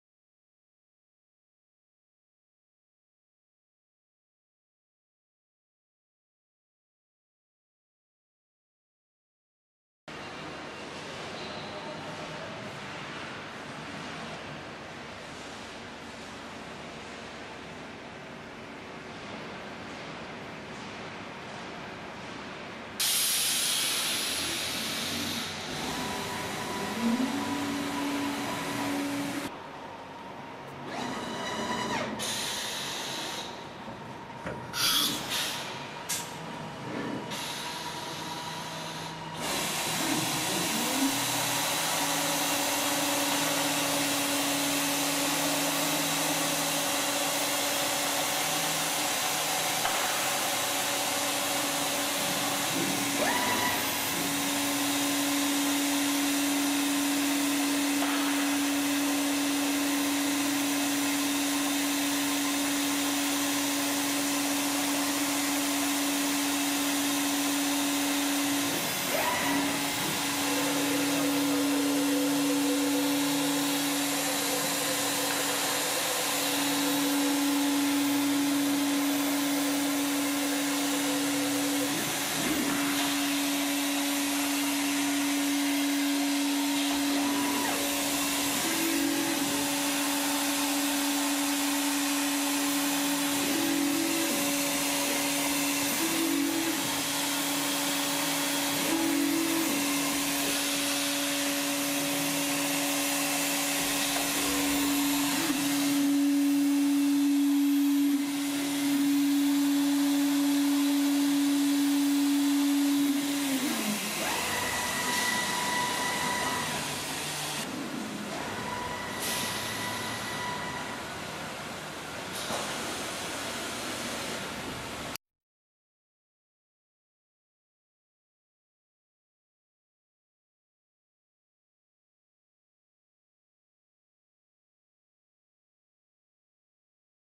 MECAL: "NIKE" 4 axes CNC machining centre
Mecal "MC 309 Nike" is a compact CNC machining centre to process aluminum or PVC profiles. It features 3+1 Axes to work on 3 planes and can process profiles up to 3490mm (137½") in a single load or 6980mm (275") processing it in a double step load.